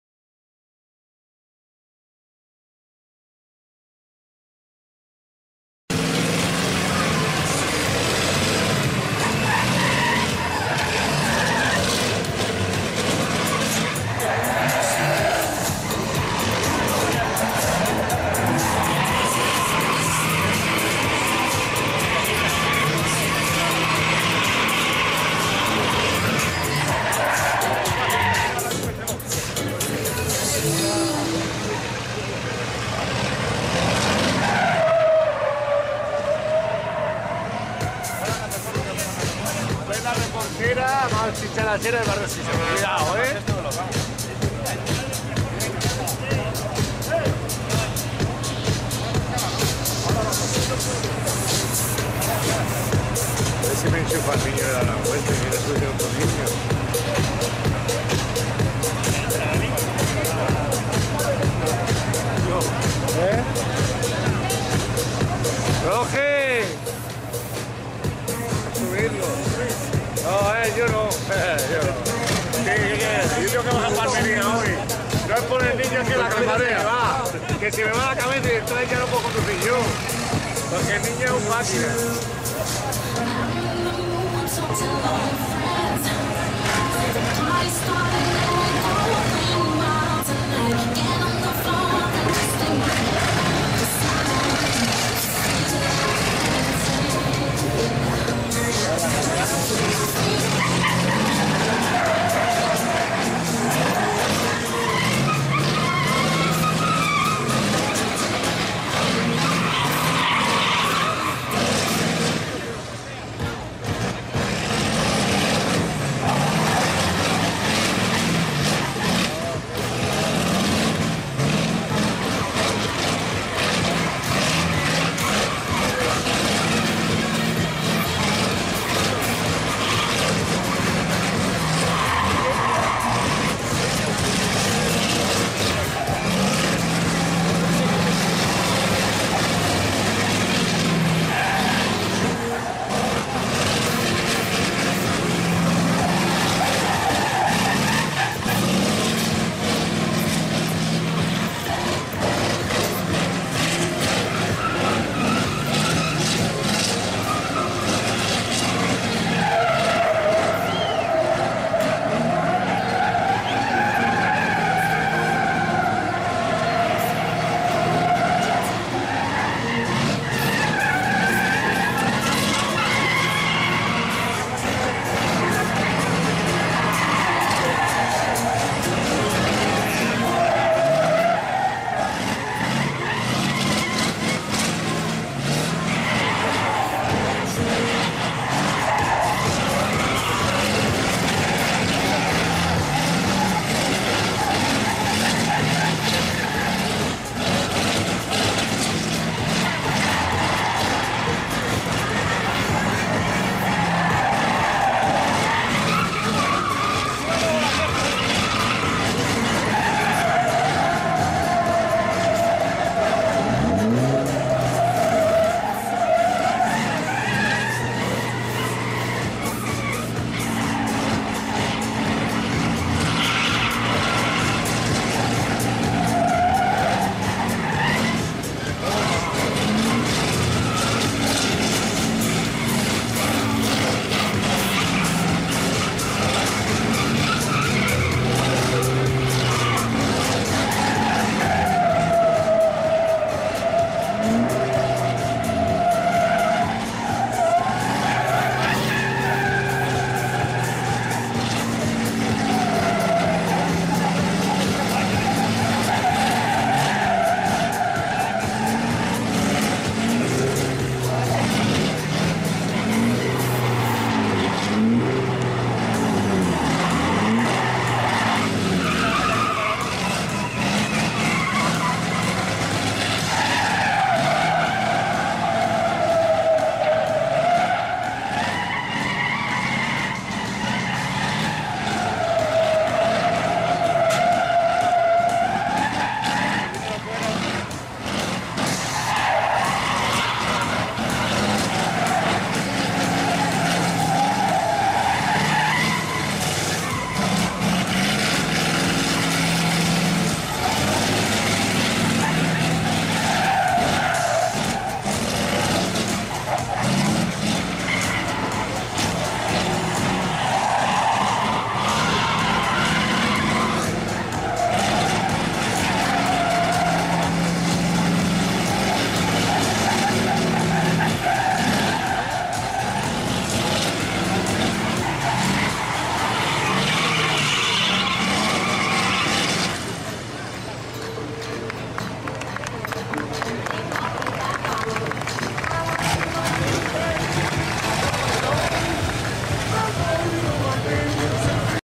ALBERTO VILA - JOSY PONFERRADA 2016
En la fiesta de San Cristobal 2016 en Ponferrada, Alberto Vila le pasa los mandos del camión a su hijo Josy Vila.